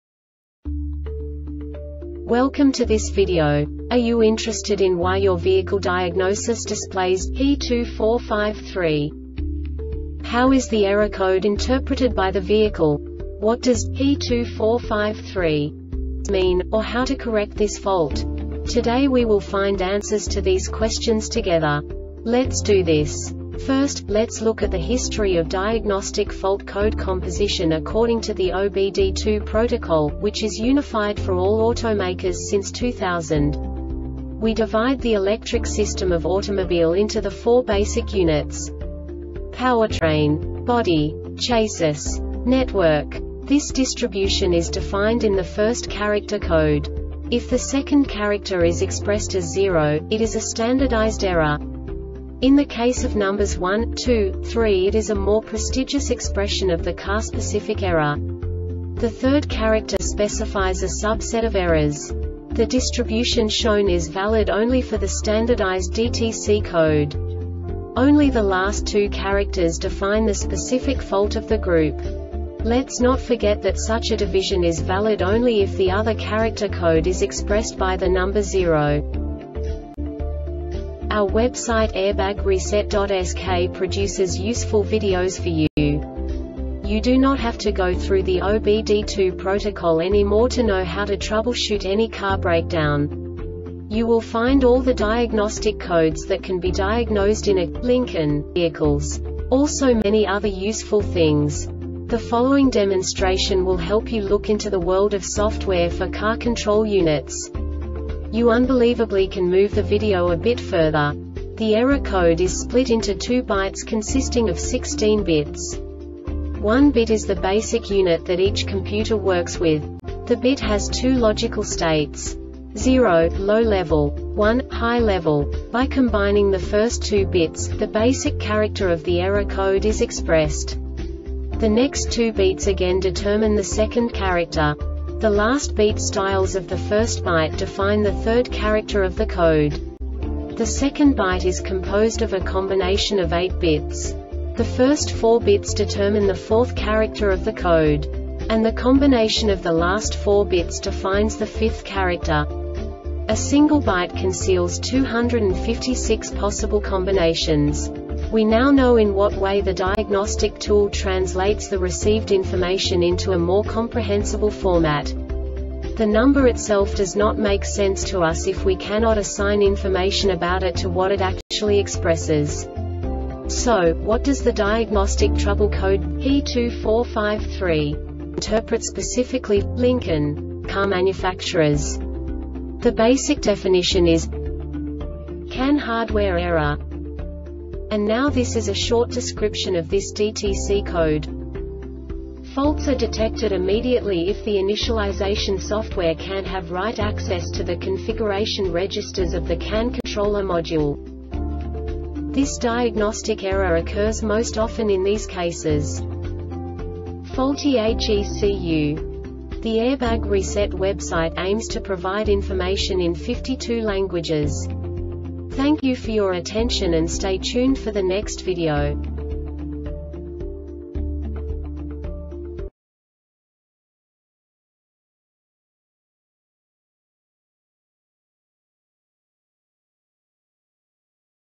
DTC Lincoln P2453 Short Explanation
Contents:
0:21 Basic DTC analysis according to OBD2 protocol standard.
1:48 Insight into programming
2:58 A diagnostically understandable description of the Diagnostic Trouble Code
3:05 Basic error definition

Make:
Lincoln
Code:
P2453
Definition:
CAN Hardware Error
Description:
Faults are detected immediately if the initialization software cant have write access to the configuration registers of the CAN-controller module.
Cause:
 Faulty HECU